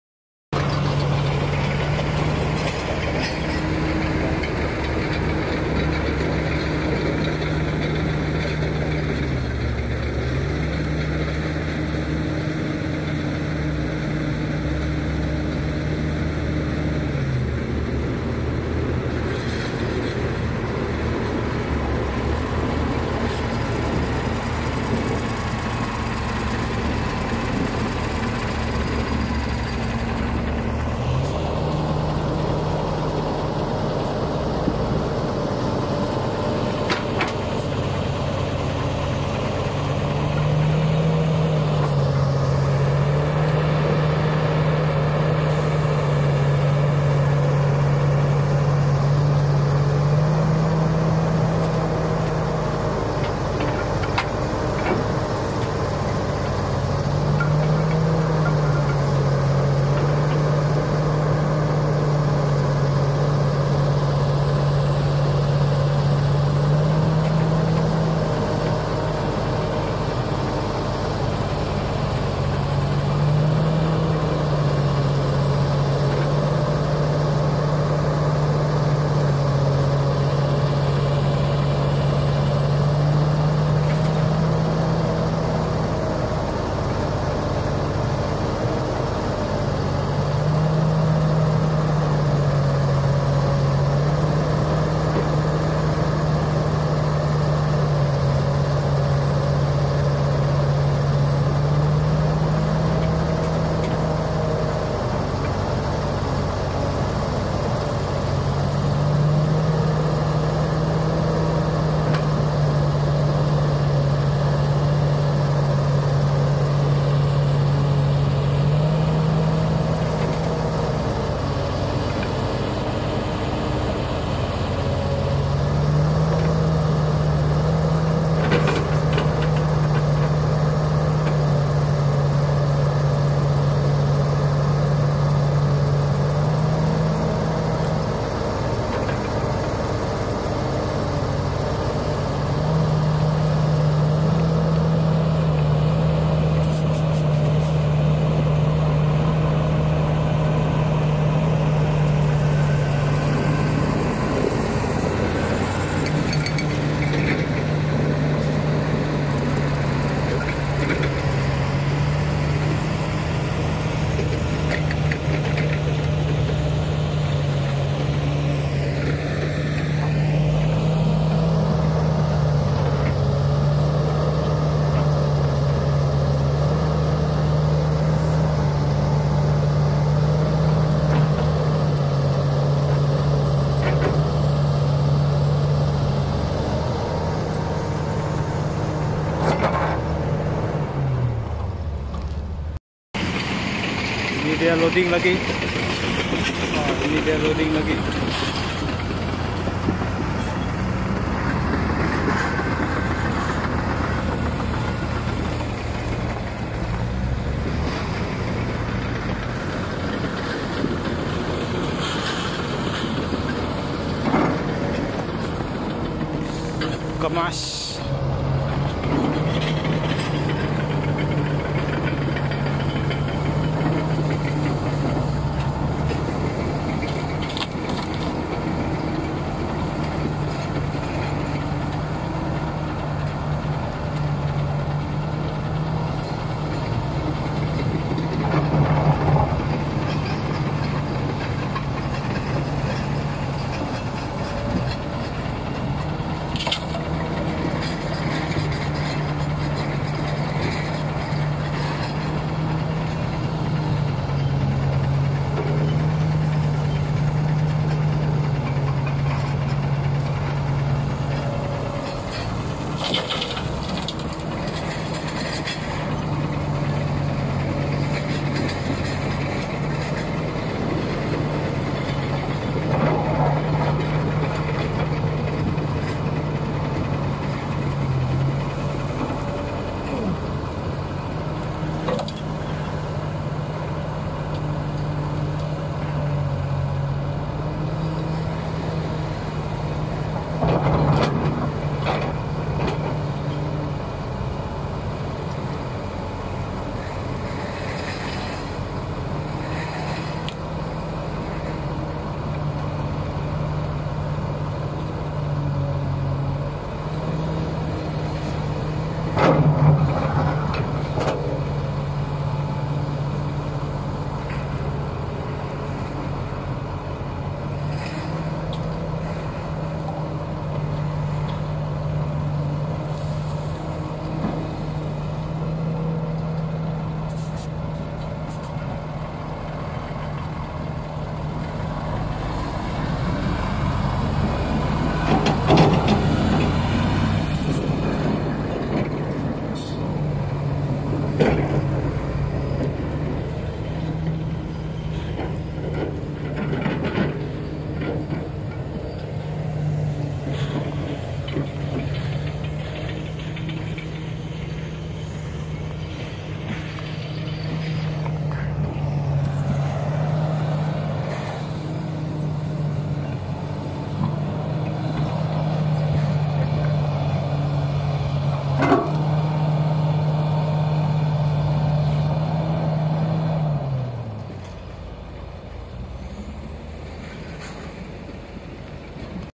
Ways of excavator loading snow garbage to Kamaz truck
Selamat datang ke nassir channel🎉🎉🎉

Cara excavator loding sampah snow ke Kamaz truck
Jangan lupa lihat video nassir channel lain sampai habis terima kasih semua 🙏🙏🙏🤝🤝